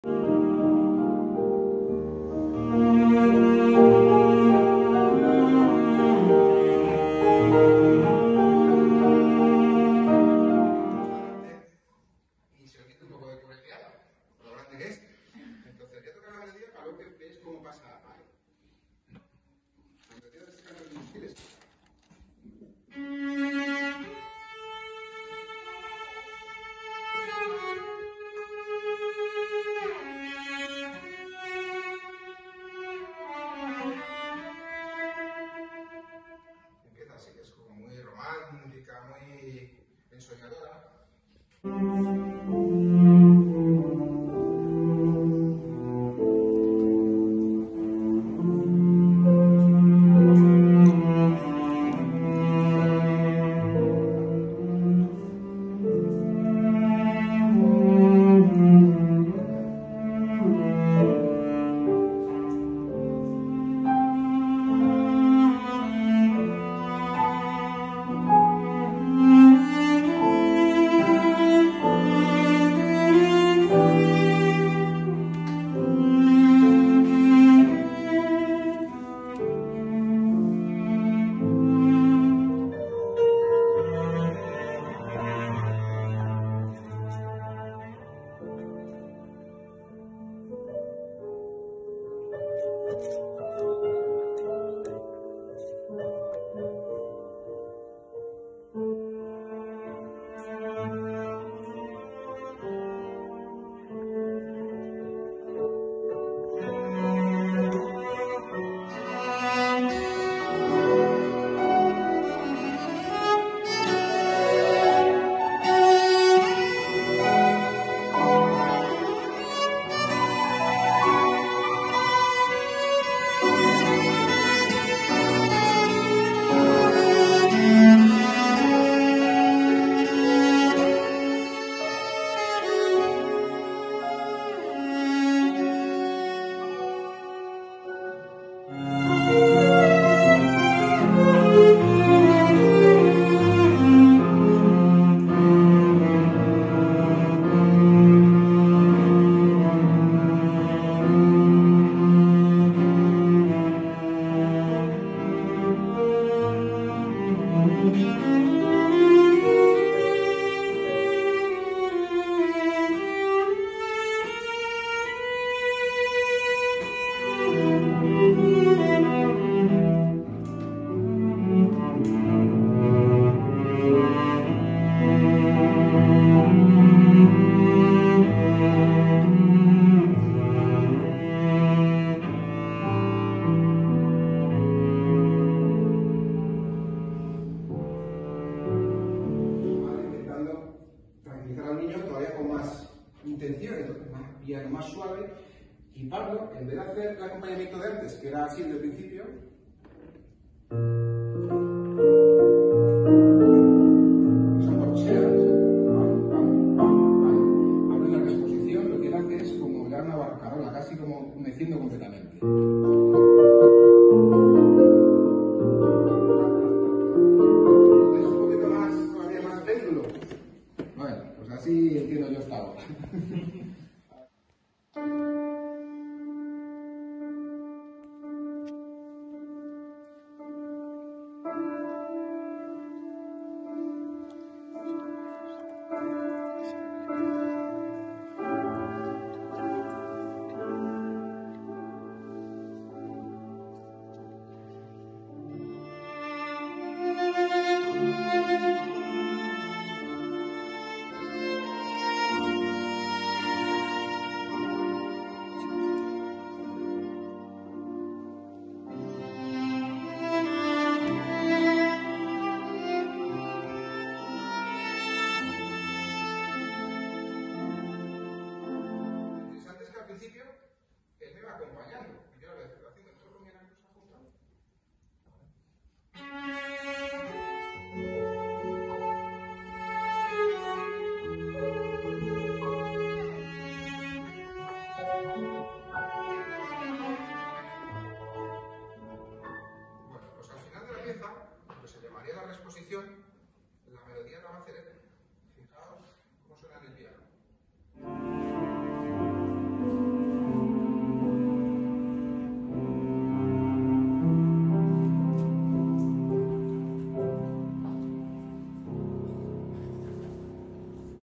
Música de cámara comentada
Nuevo proyecto junto a Alberto Hernández (violonchelo). Recitales comentados.
El evento ideal. Música de cámara en un ambiente desenfadado.
Interesados, contactar por privado